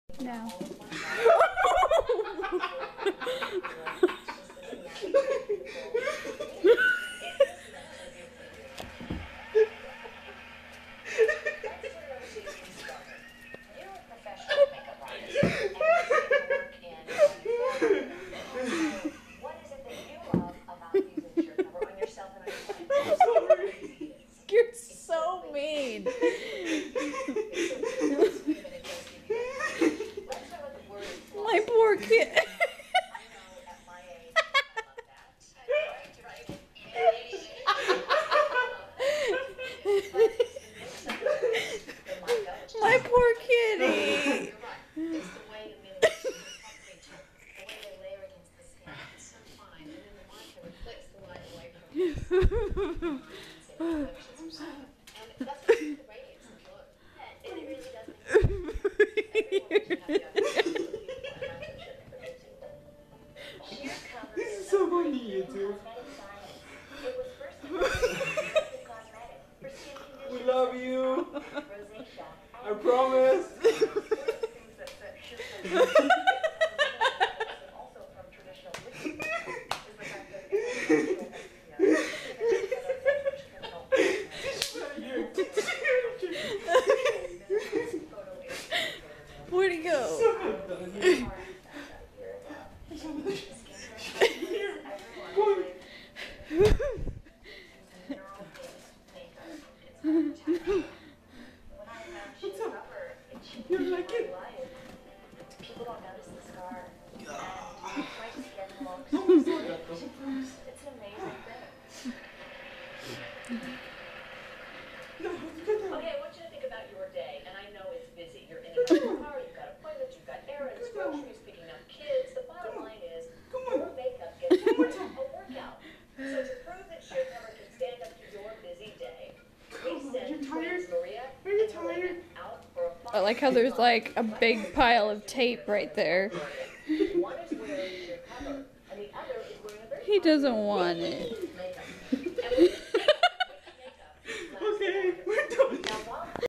Funny Cat Tape On Paws
my kitty ALVIN with tape in his little paws! omg it was hilarious we love you gatito :)